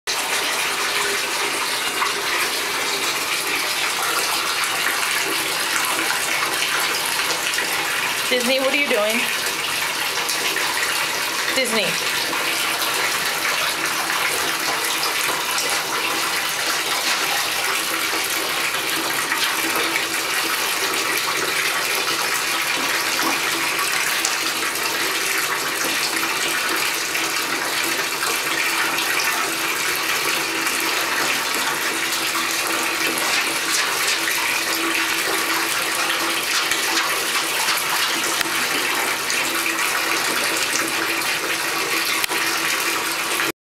Disney takes a bath!
Disney, a Pomeranian rat terrier mix, loves water! He paws at the drain in the tub to close it so the tub will fill up with water.